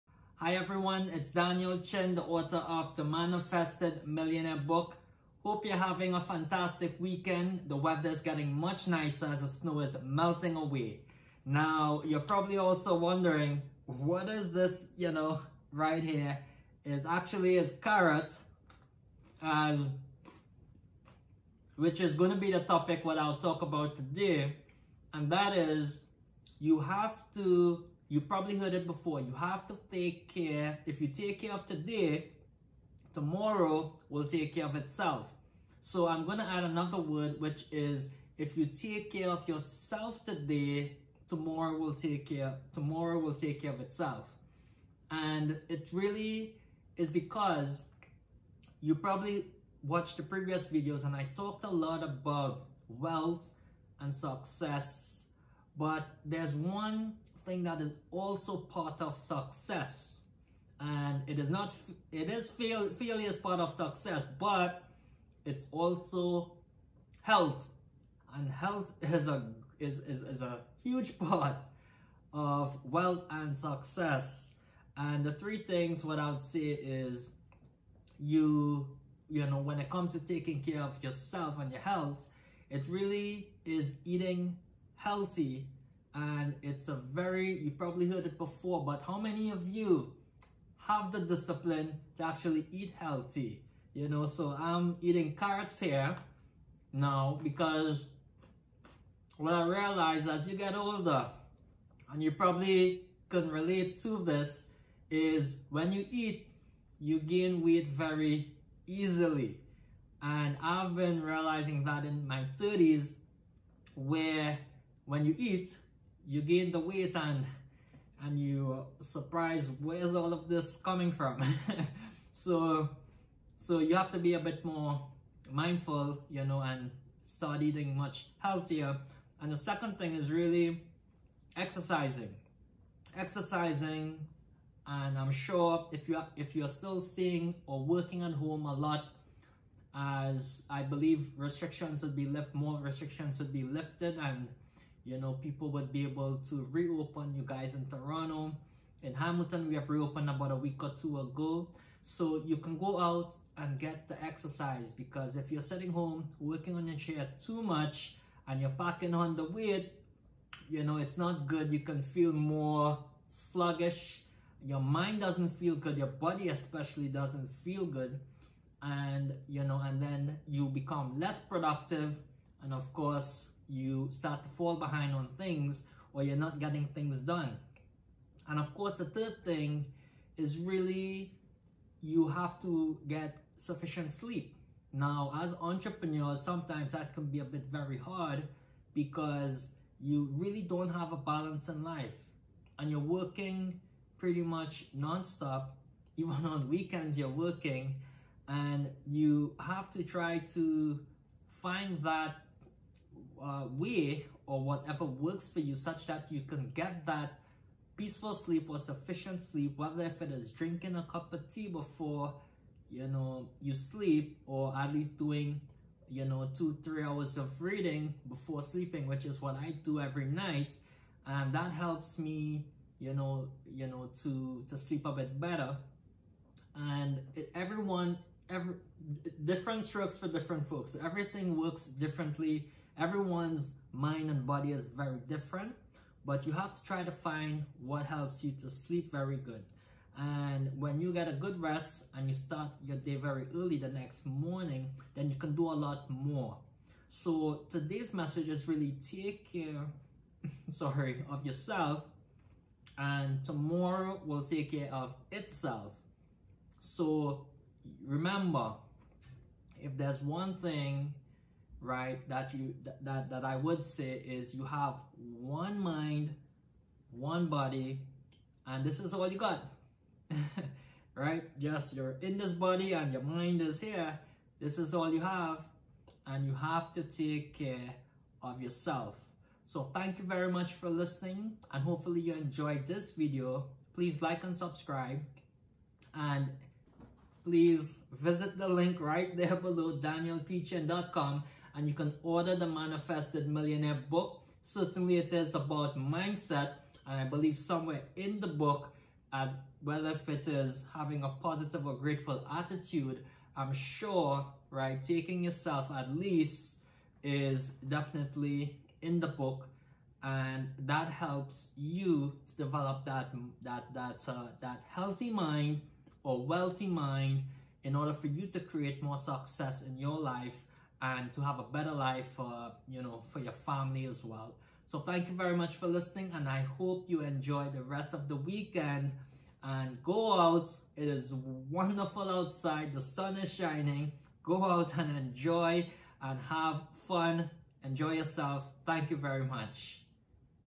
3 Self-care tips during COVID-19
3 Self-care tips during COVID-19:

1) Eat healthy

2) Sleep well

3) Exercise

“Take care of yourself today, and tomorrow will take care of itself”